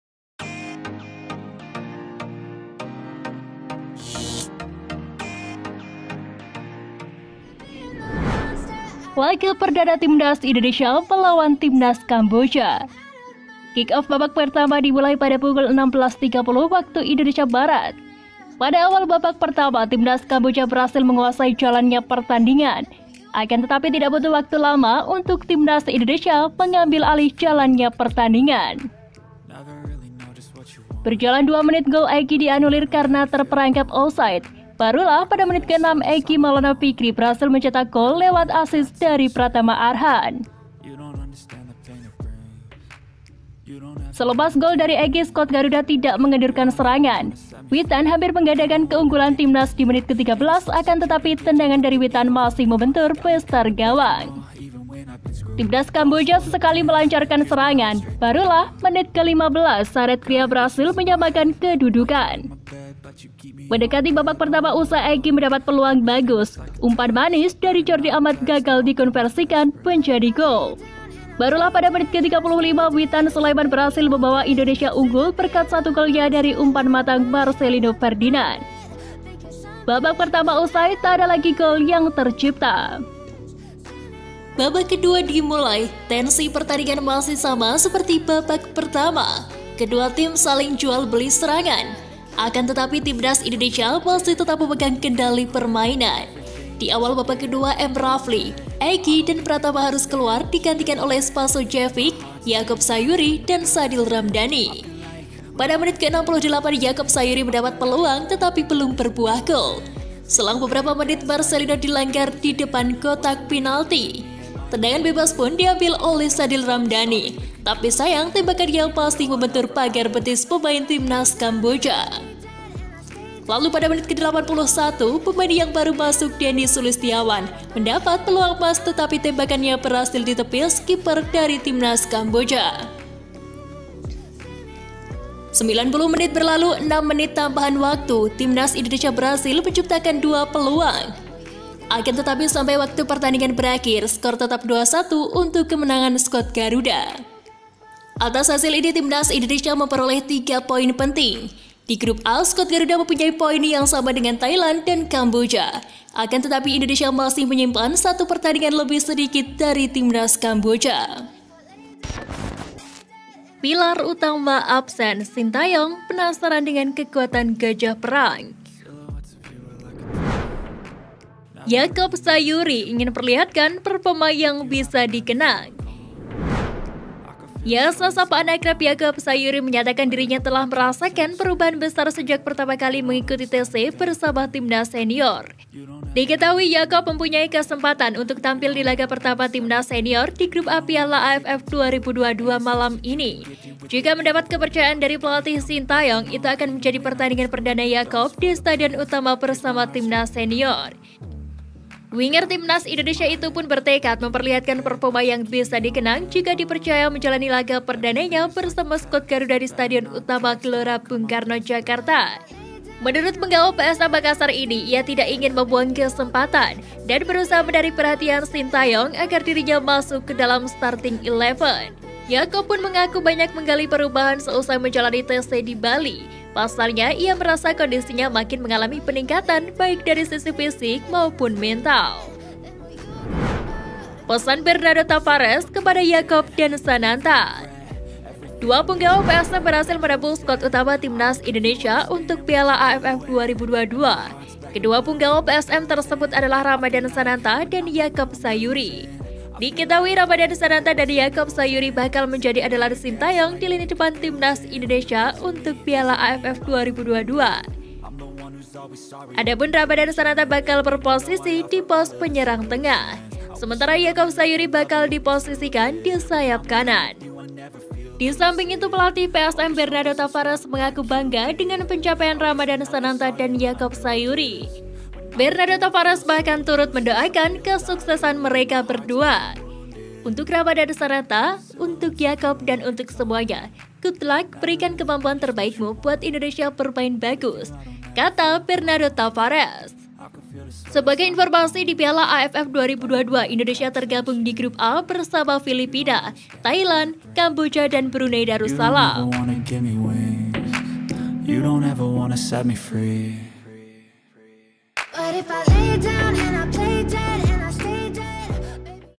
Indonesia Tekuk Kamboja| Yakob Sayuri Tampil Apik| Tavares Beri Wejangan Yakob dan Sananta
BERITA BOLA TERBARU DAN JUGA HASIL PERTANDINGAN TADI MALAM DARI FAR PLAY SPACE - Berita Bola : Indonesia Tekuk Kamboja| yacob sayuri Tampil Apik| Tavares Beri Wejangan Yacob dan Sananta

Tetap jaga kesehatan, semoga dilancarkan rezeki dan segala urusan. Salam hormat dari seluruh admin Fair Play Space :)